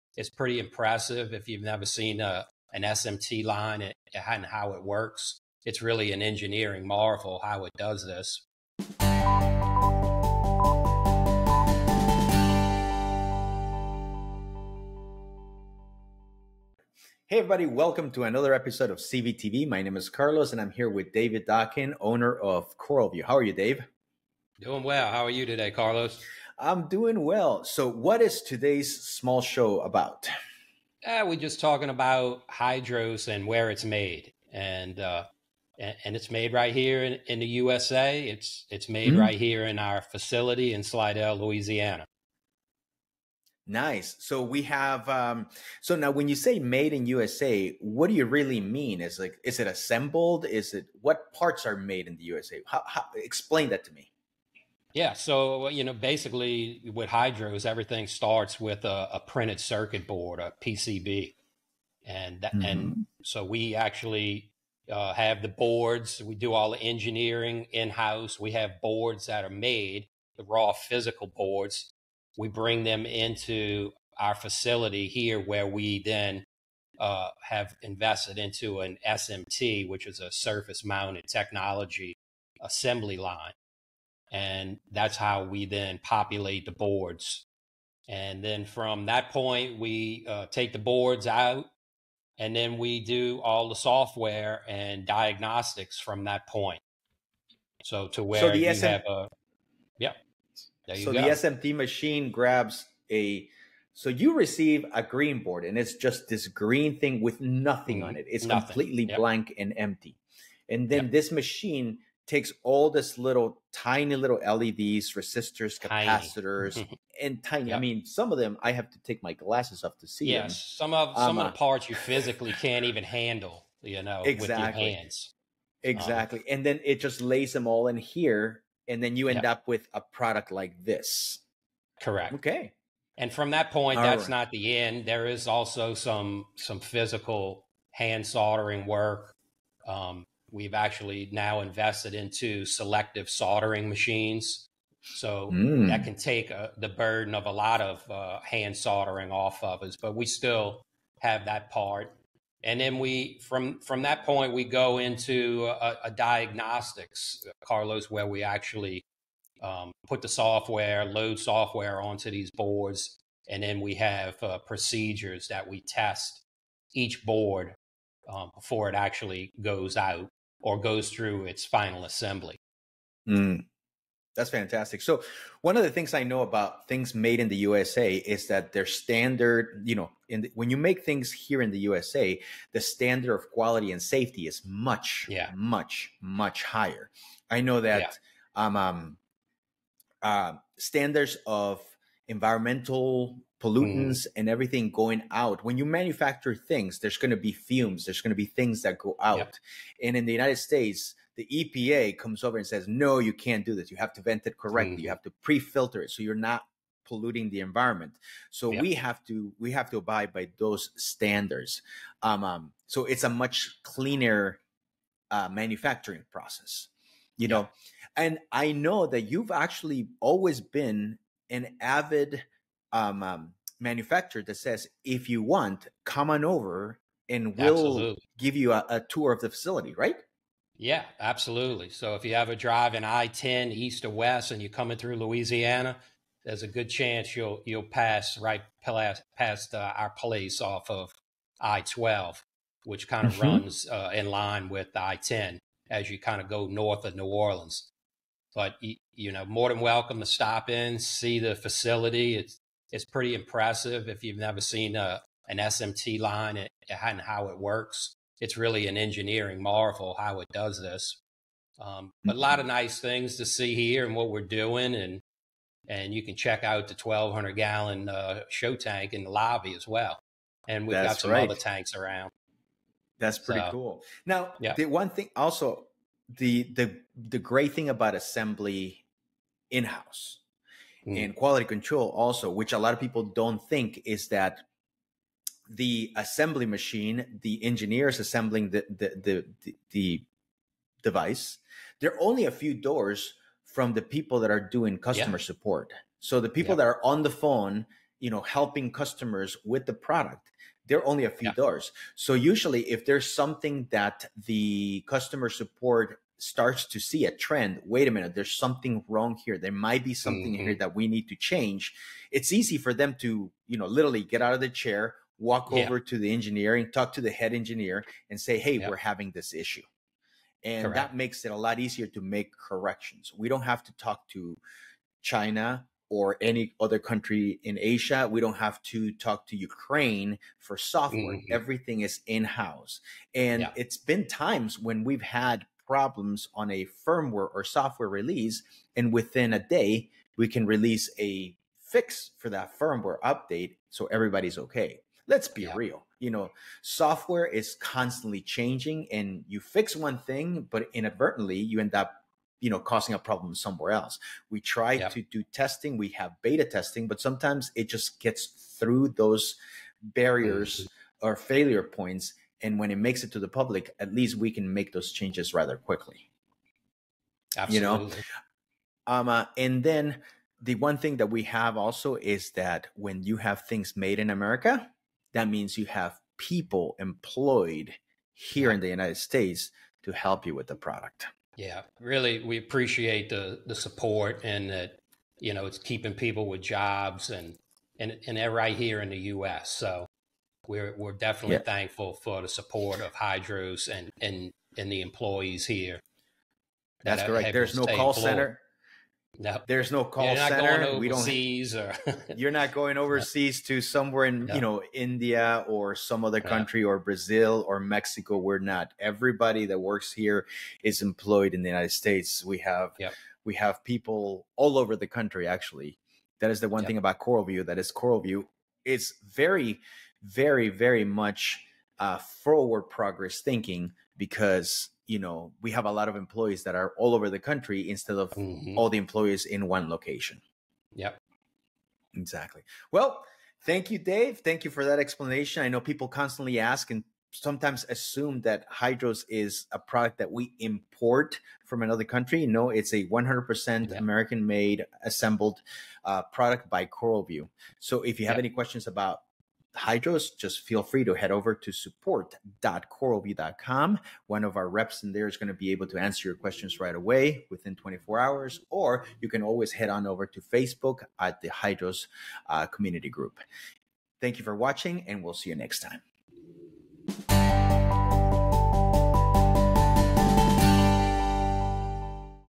HYDROS Aquarium Controller is Made in the U.S.A.
In this episode of CVtv, hosts David D'Aquin and Carlos Chacon take us behind the scenes at CoralVue headquarters in Slidell, Louisiana to show us how HYDROS, a cutting-edge aquarium controller system, is made in the USA. From the design process to the assembly line, we learn about the skilled work that goes into creating this innovative product.